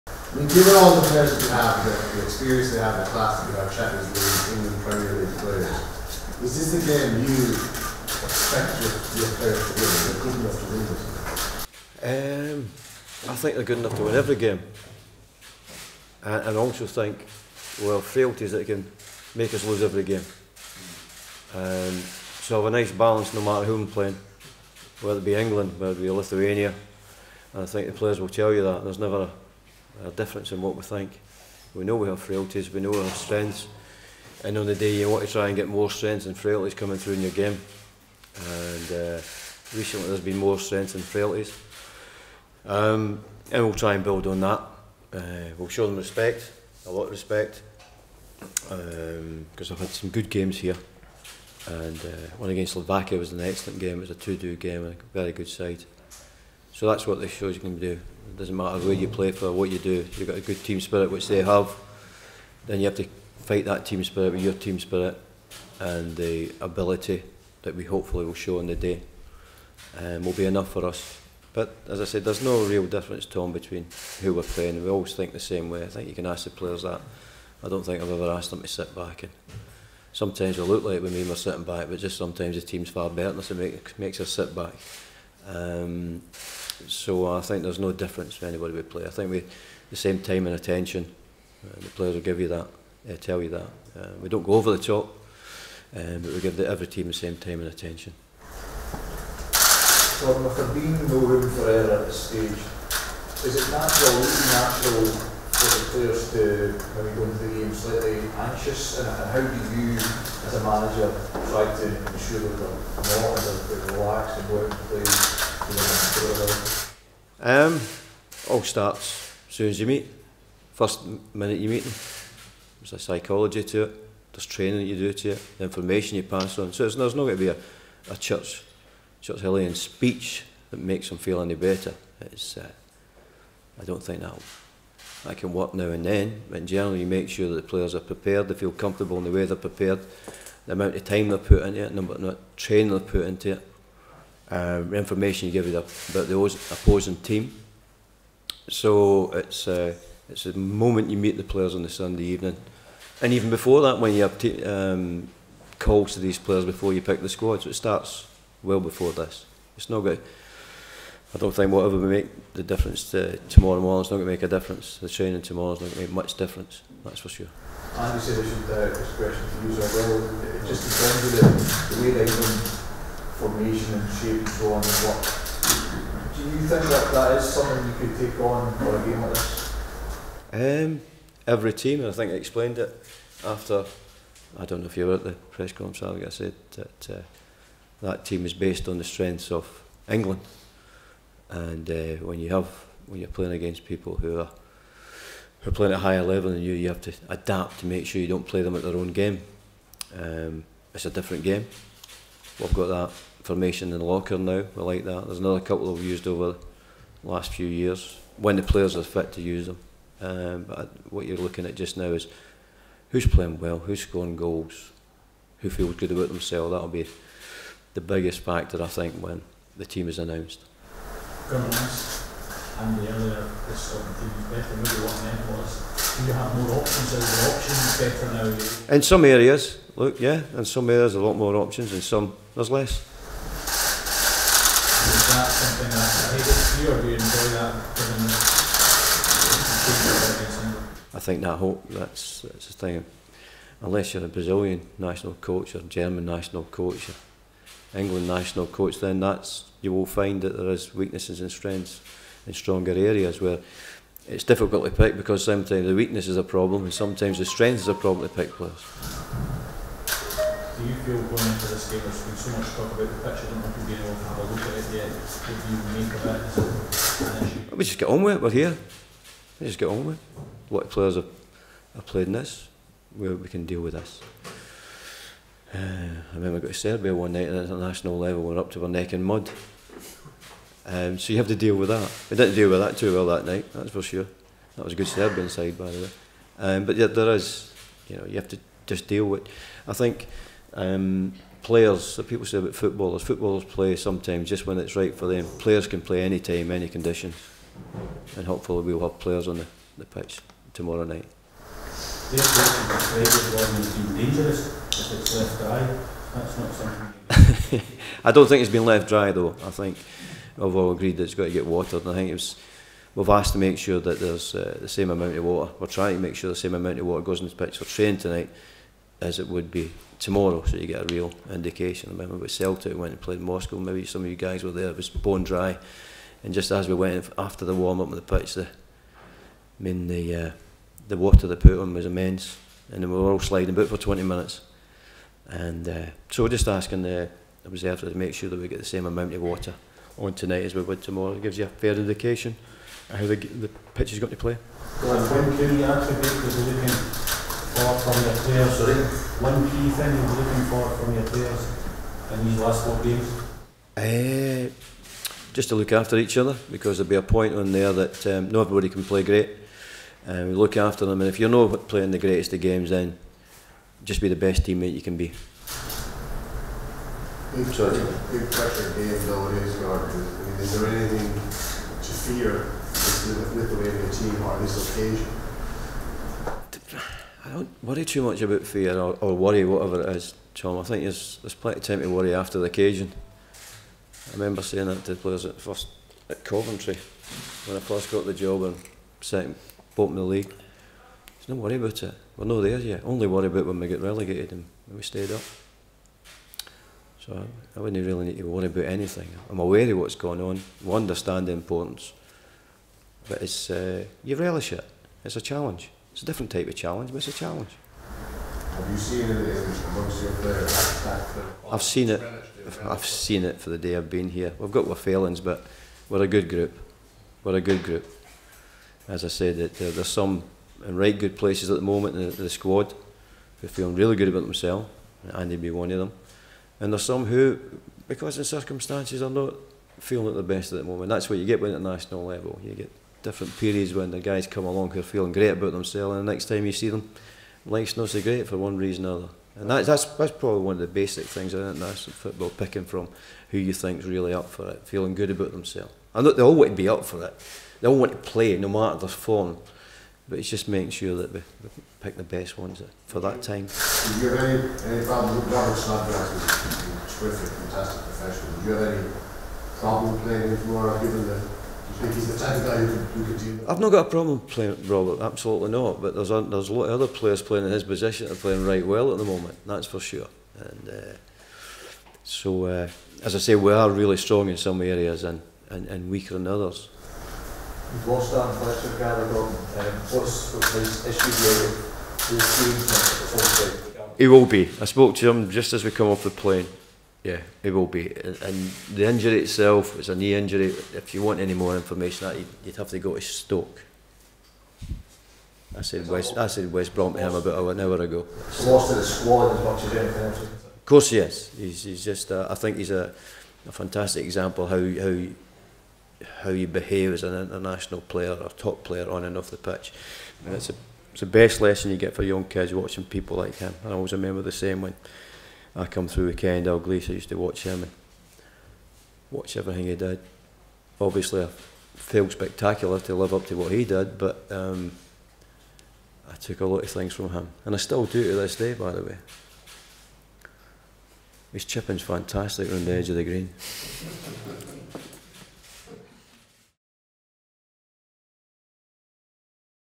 Lithuania v Scotland | Gordon Strachan's Pre-Match Press Conference.
Gordon Strachan spoke to the media ahead of Scotland's World Cup Qualifying match against Lithuania in Vilnius.